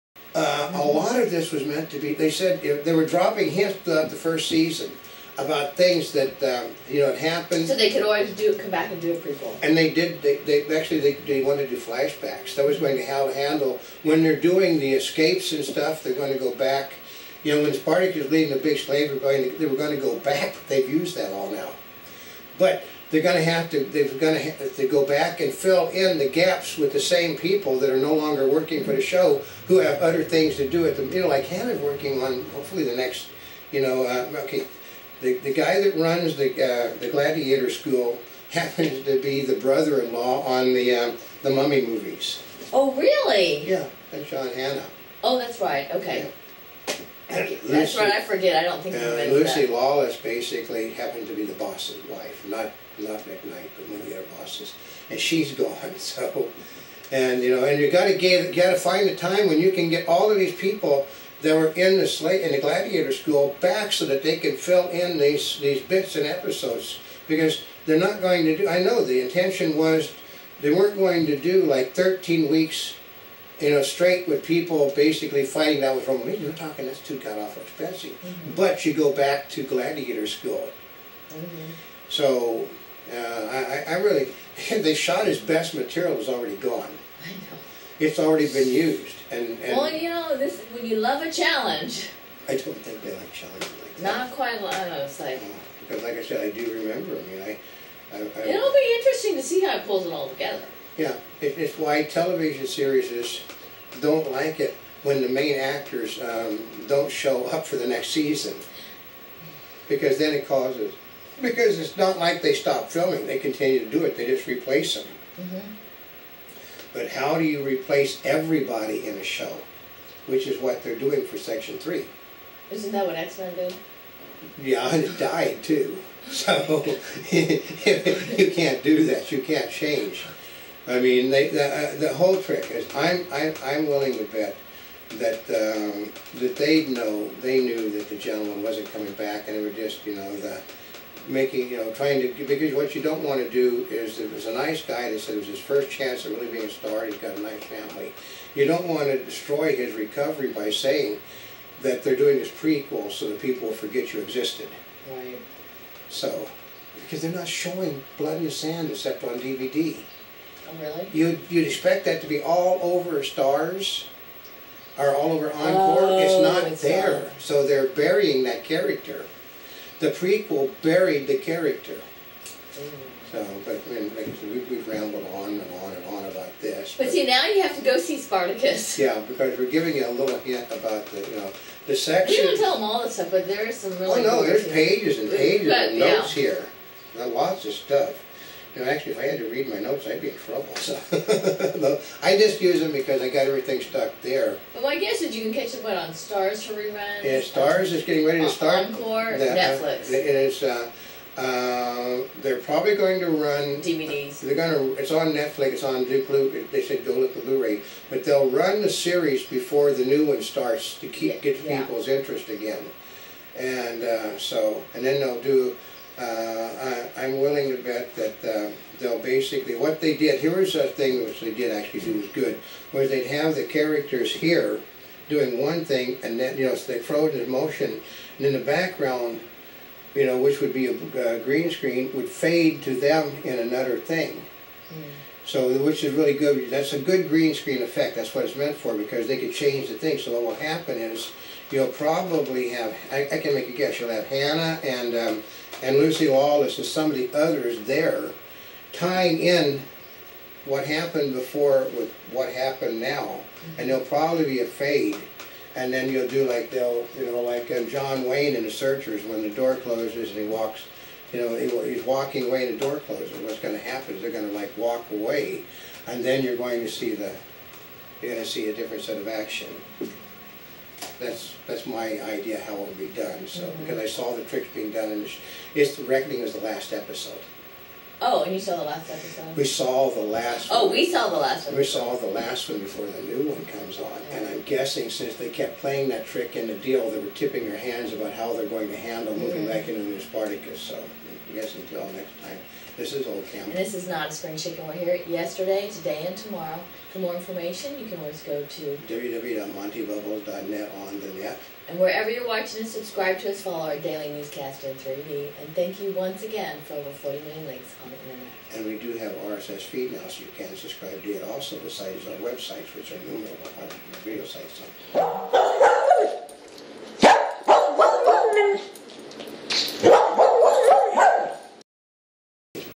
Emmy Screening of Spartacus: Gods of the Arena review final part in 3D
Oldecam and Notaspringchick for Newsvideoweb with final part of their review of the Wraps Emmy Screening of Spartacus: Gods of the Arena' with Q&A with Steven DeKnight in anaglyph in 3D,